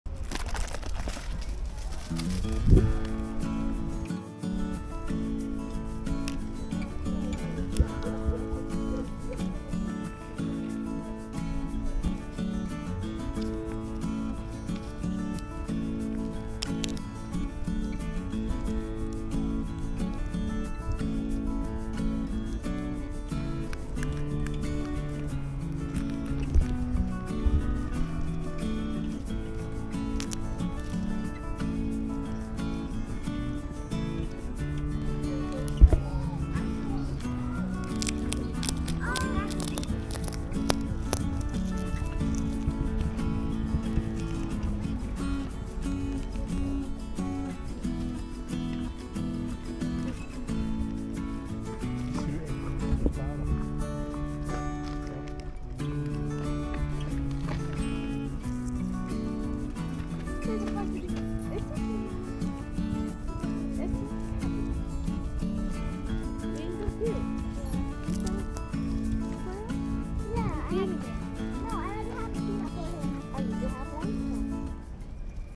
Cute fluffy
Cute fluffy squirrel withMe.
This cheerful squirrel likes to treat himself to nuts from my hands.